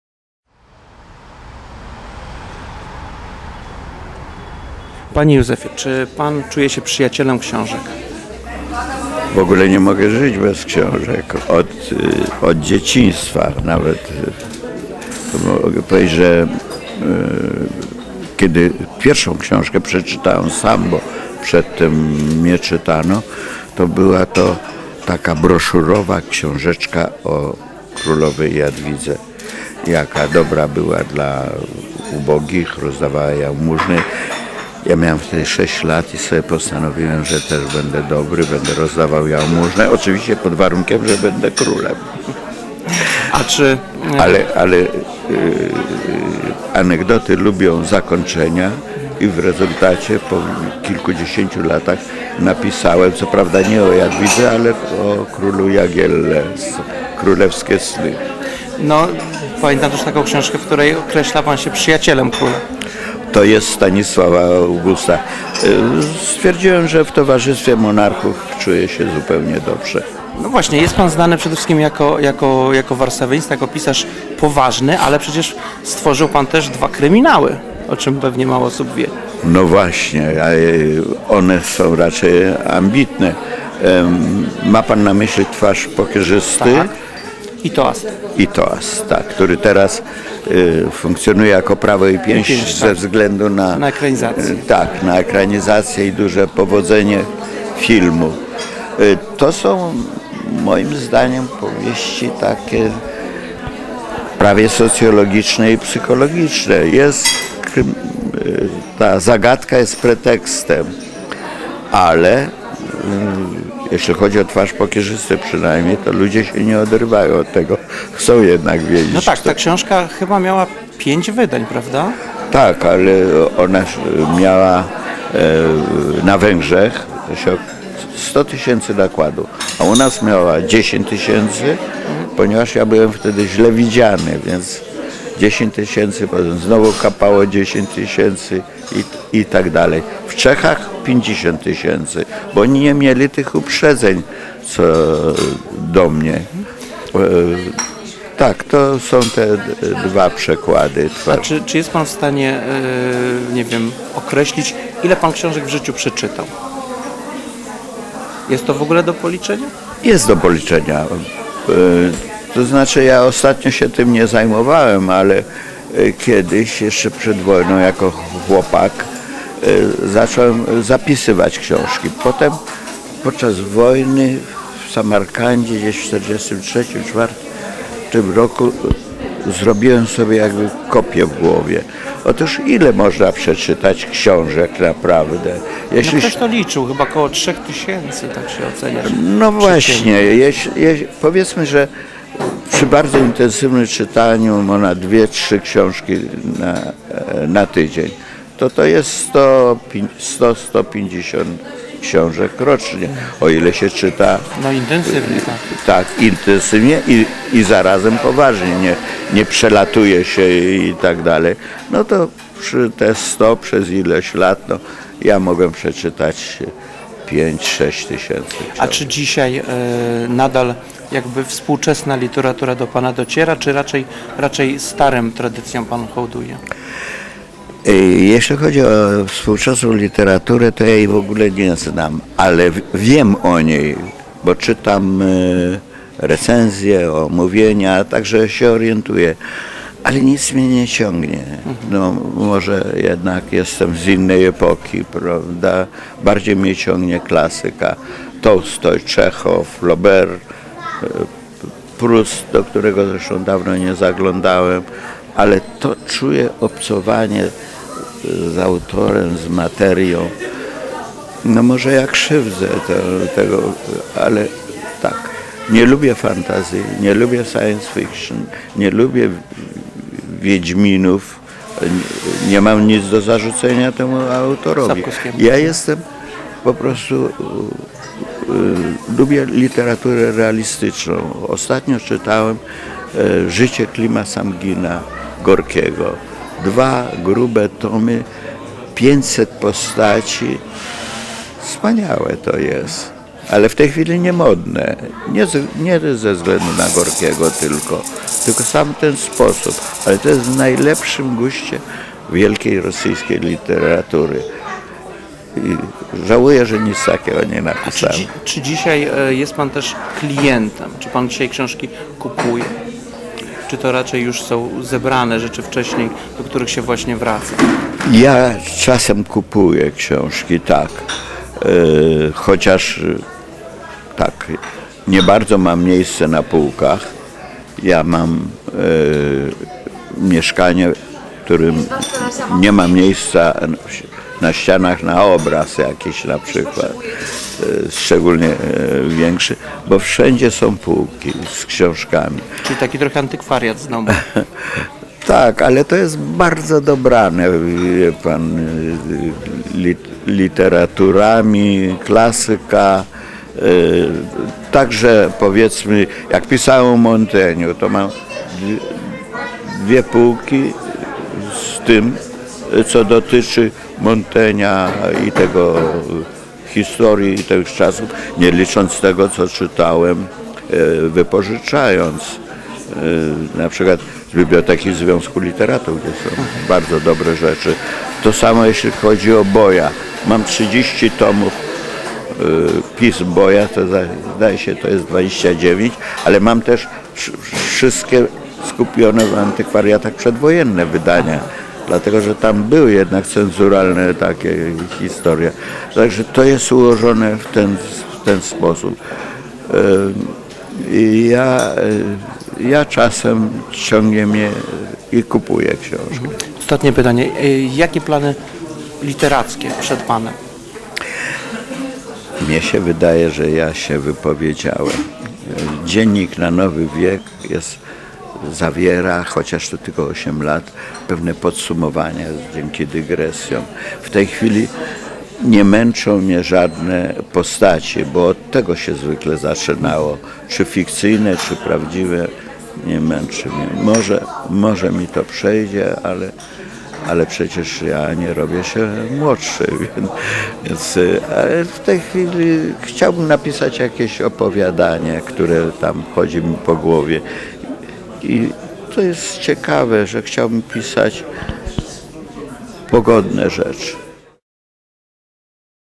Józef Hen - wywiad - Książki z Nowolipek
I Muranowski Kiermasz Książki

18 września 2010r.

nagranie i realizacja: Piotr A. Latos